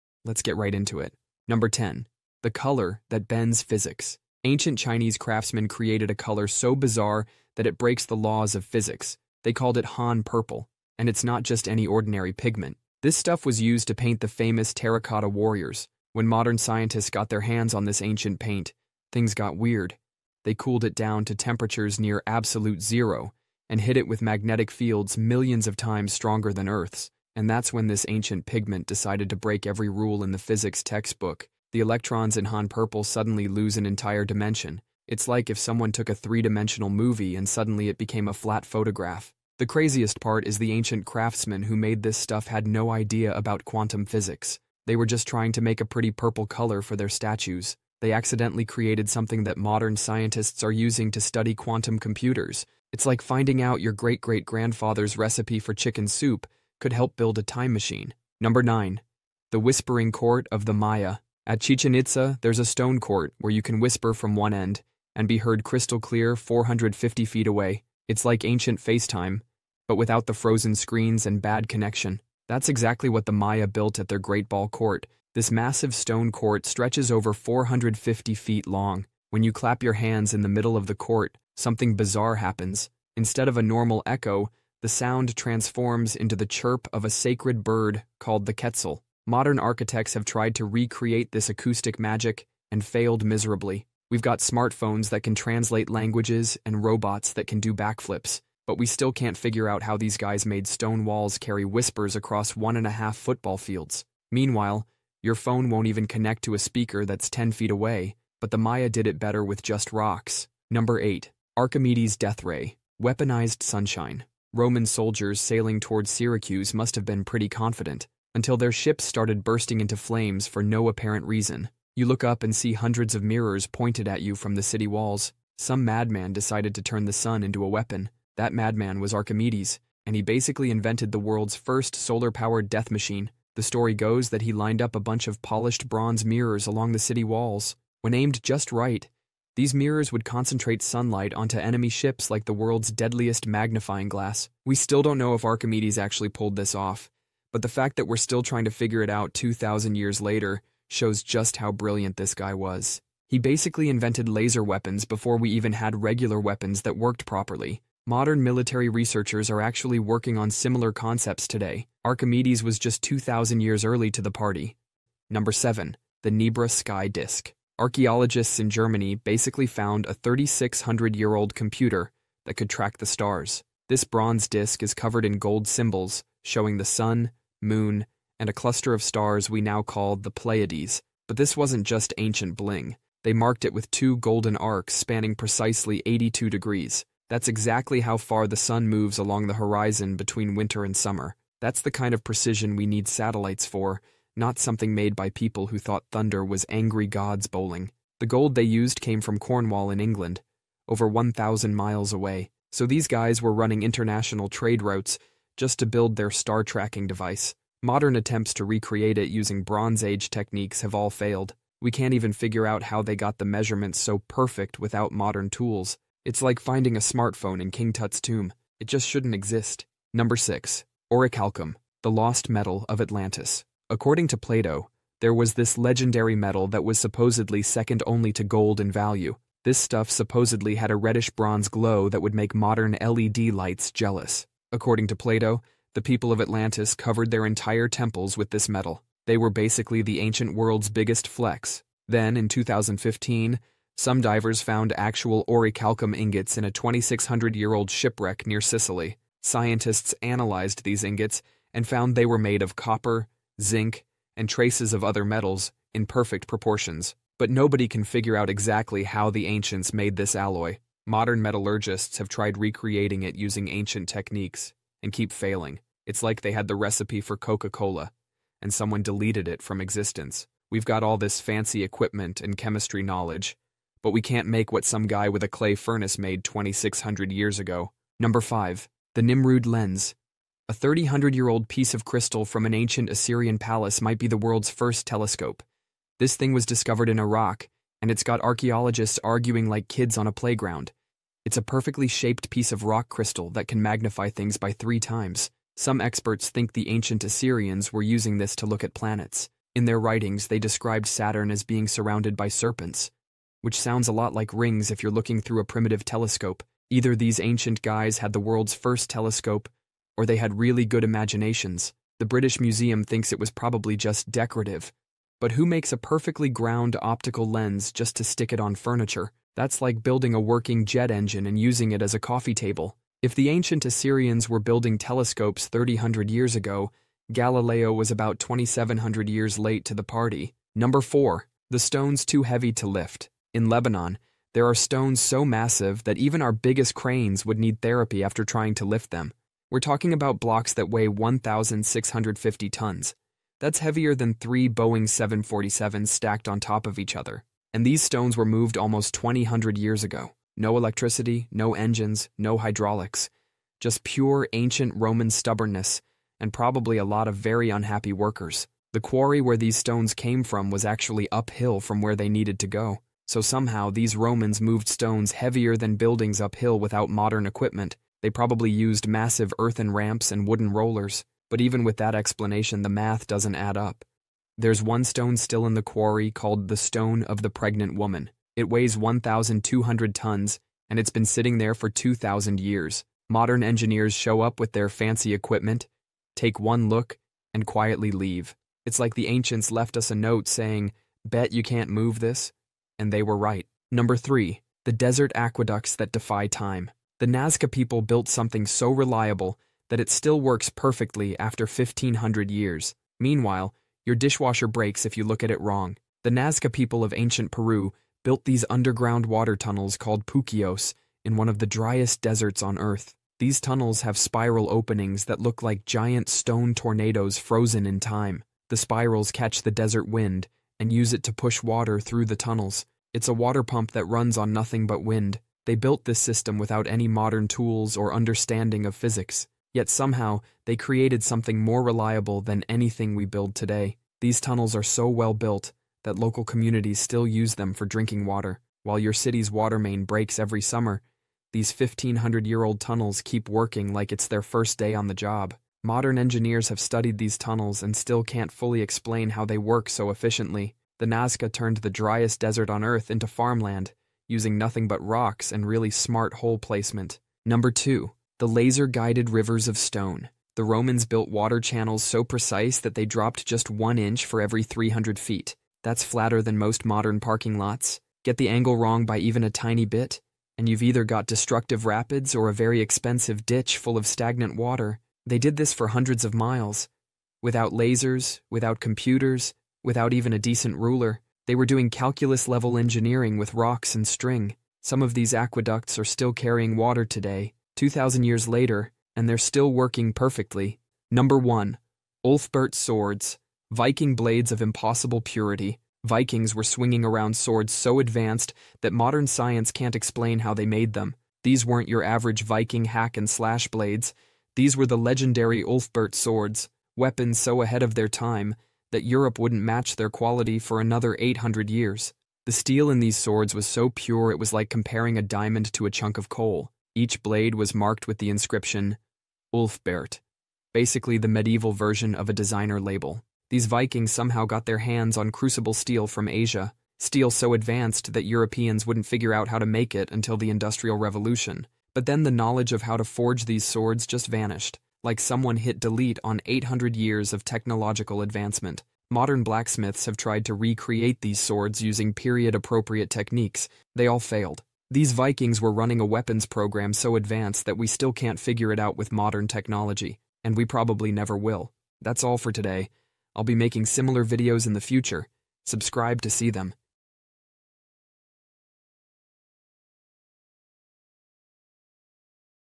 Ancient Inventions That Are Still Impossible
00:00 The Color That Bends Physics
00:58 The Whispering Court of the Maya
01:50 Archimedes' Death Ray: Weaponized Sunshine
02:45 The Nebra Sky Disk
03:40 Orichalcum: The Lost Metal of Atlantis
04:38 The Nimrud Lens
05:30 The Stones Too Heavy to Lift
06:33 The Desert Aqueducts That Defy Time
07:37 The Laser-Guided Rivers of Stone
08:11 Ulfberht Swords: Viking Blades of Impossible Purity